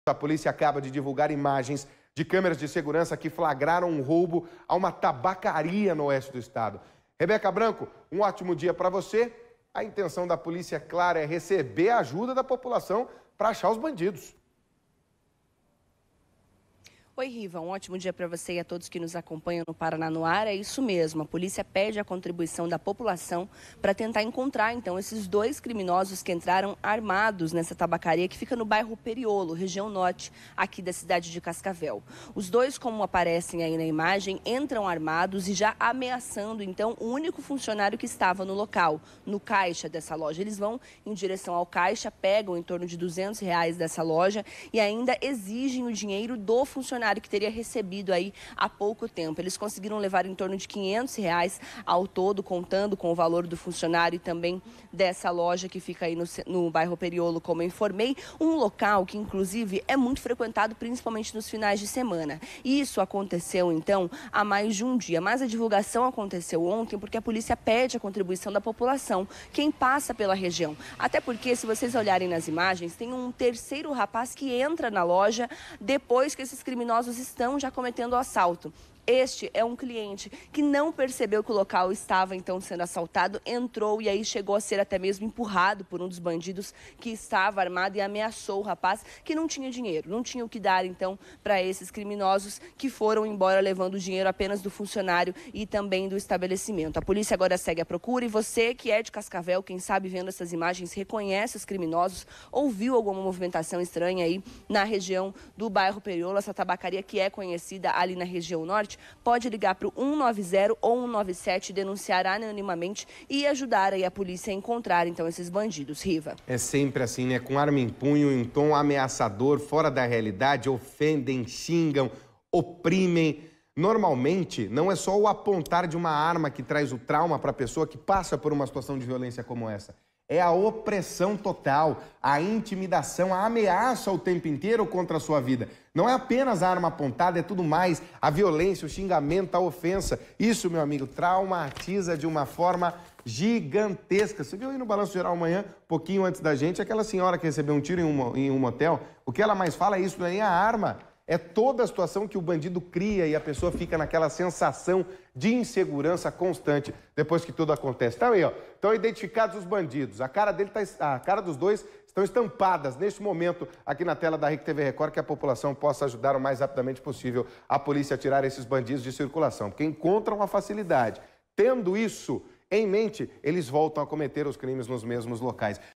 Polícia divulga imagens do roubo de tabacaria em Cascavel
A Polícia tem a intenção de obter ajuda da população para identificar os criminosos que assaltaram uma tabacaria de Cascavel. Confira!